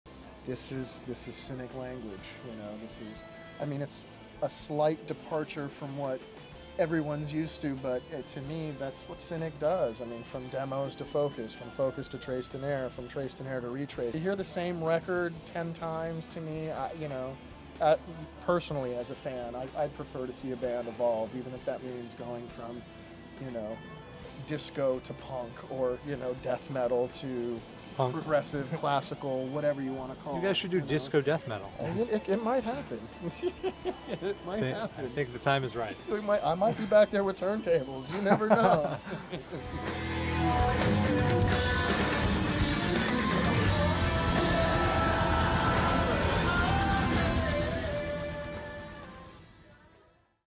Cynic Paul & Sean Interview Variability of Cynic
highlight clip video from Cynic Paul & Sean interview